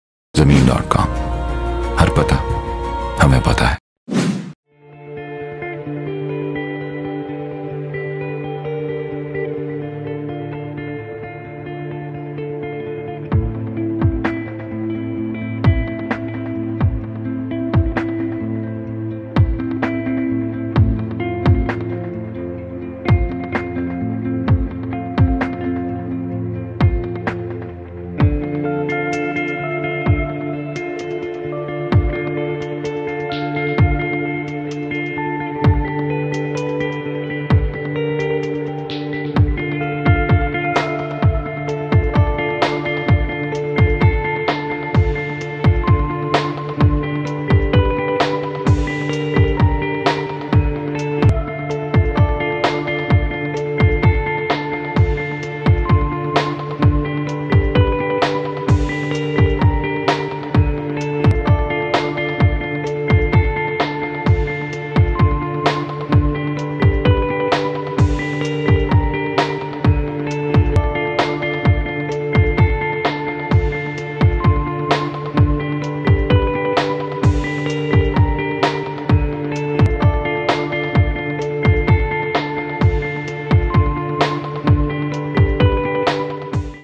5 MARLA RESIDENTIAL PLOT AVAILABLE FOR SALE IN PHASE 3 GARDEN TOWN GUJRANWALA
If you are searching for an ideally located 5 Marla Residential Plot for sale at a reasonable price of Rs 3,000,000, we have just the opportunity for you. The 5 Marla Residential Plot is located in Garden Town, Garden Town, Gujranwala.